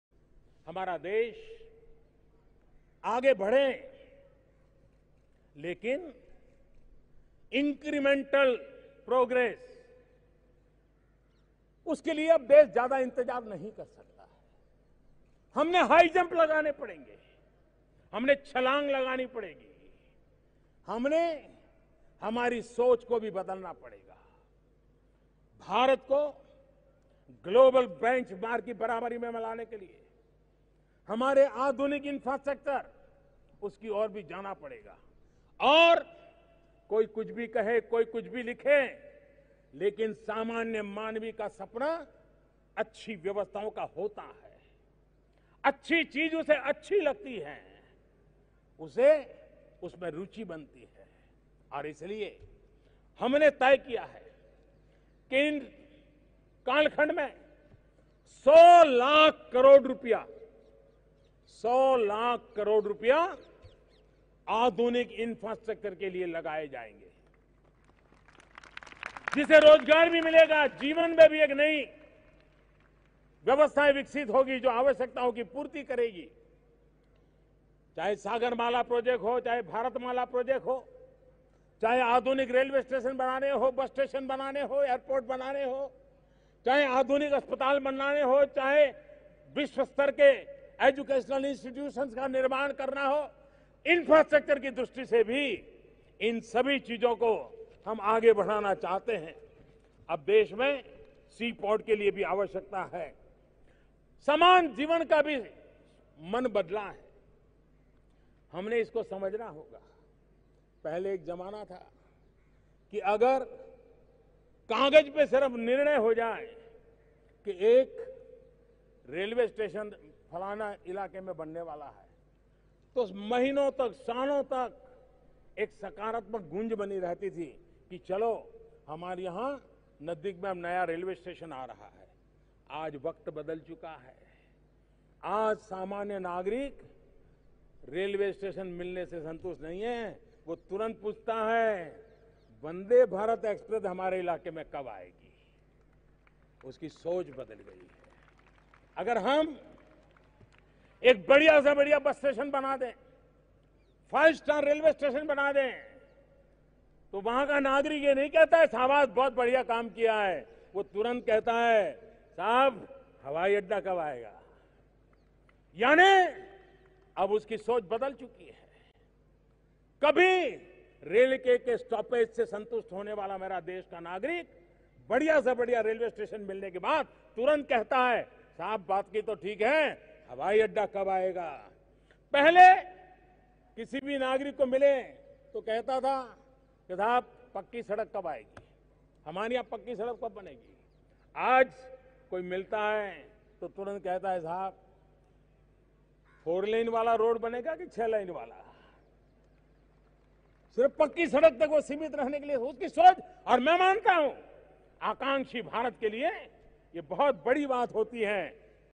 100 लाख करोड़ रुपए आधुनिकीकरण के लिए लगाए जाएंगे PMModi
पीएम मोदी ने कहा कि आज देश में 21वीं सदी की आवश्यकता के अनुसार आधुनिक इंफ्रास्ट्रक्चर का निर्माण हो रहा है...  देश के इंफ्रास्ट्रक्चर पर 100 लाख करोड़ रुपए का निवेश करने का फैसला किया गया है...